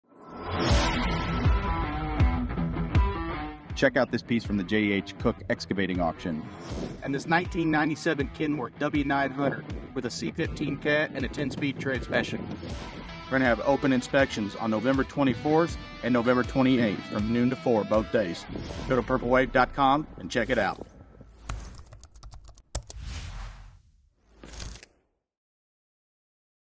J.H. Cook Construction KW + Purple Wave Auction
Check out this 1997 Kenworth W900 semi truck up for auction! Don't miss your chance to bid on this beauty with NO RESERVE. Open inspection dates are on November 24th and 28th. Start placing your bids now & get ready for some exciting action!